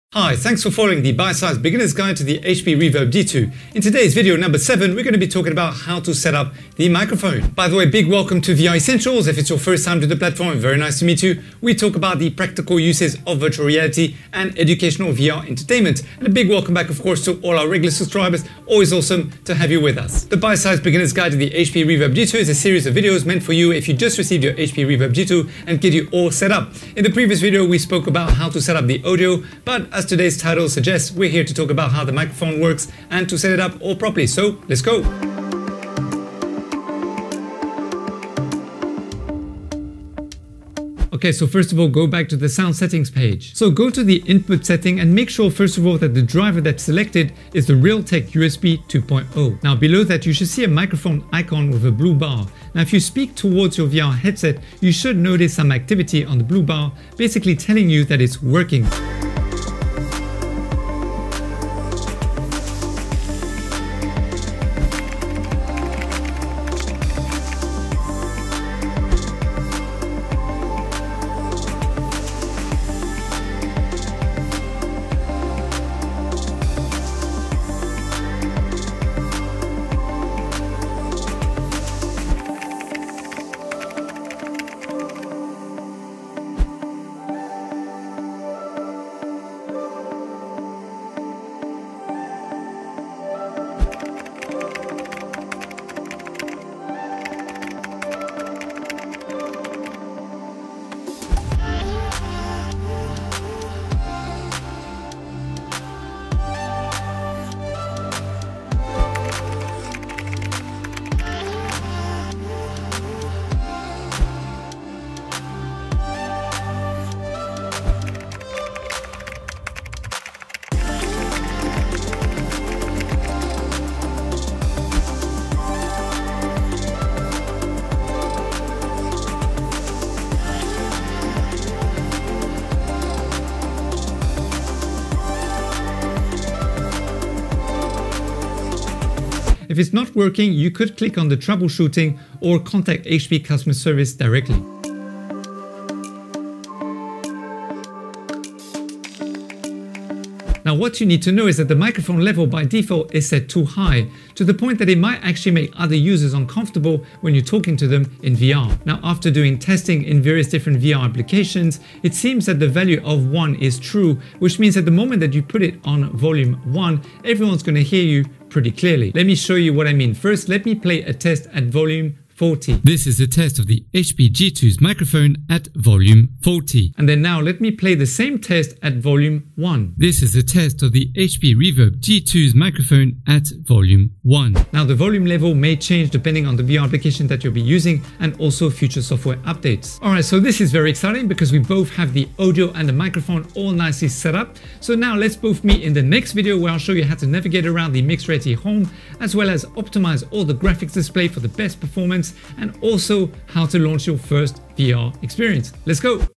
HP REVERB G2 - How to set up the microphone - tips/best practices
HP REVERB G2 - Microphone set up tips and tricks - levels & debugging

BATTERIES
1.5V rechargeable AA batteries

4K HDMI cable